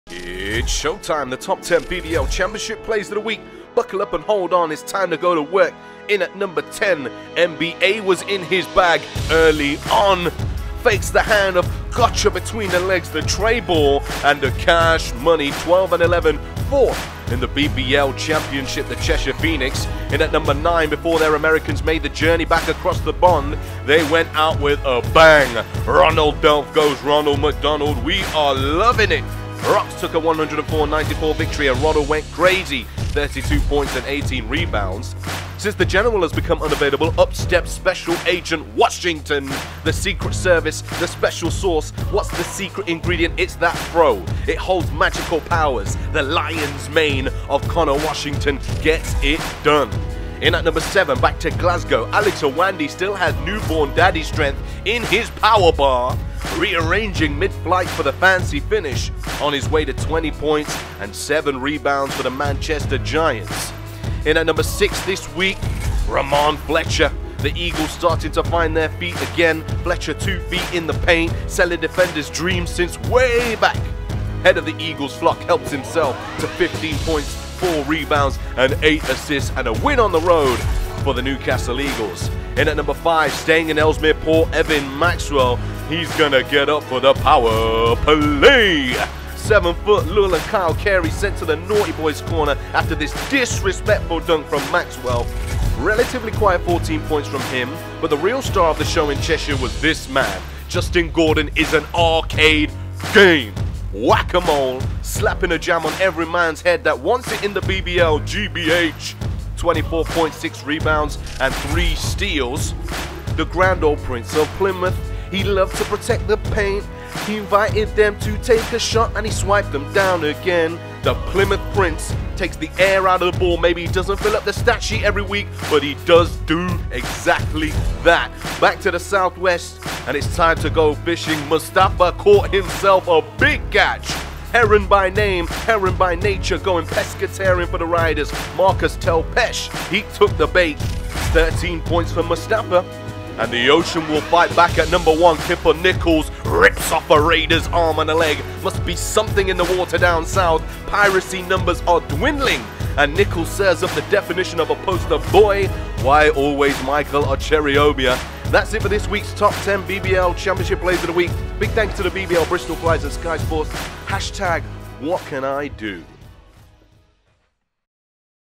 Kipper Nichols catches a body! BBL Top 10 Plays - Week 17
Take a look back on the top 10 plays from Week 17 of the BBL Championship, led by Sheffield Sharks' Kipper Nichols serving up a poster!

Also featuring: Matthew Bryan-Amaning, Ronald Delph, Conner Washington, Alex Ouandie, Rahmon Fletcher, Evan Maxwell, Justin Gordon, Prince Ibeh, Mustapha Heron,